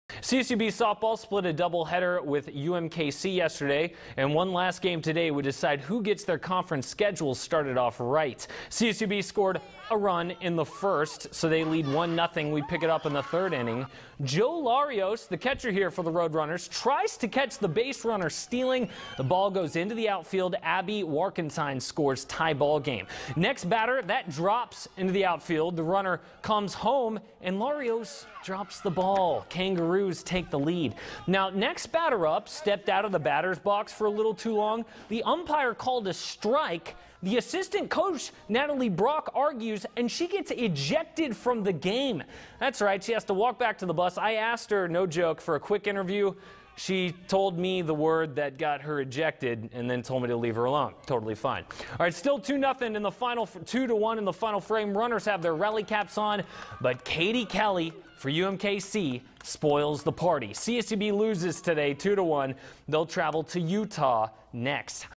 CSUB softball drops close one to UMKC, coach ejected
The Roadrunner softball team lost their third conference game of the year 2-1 at home to UMKC.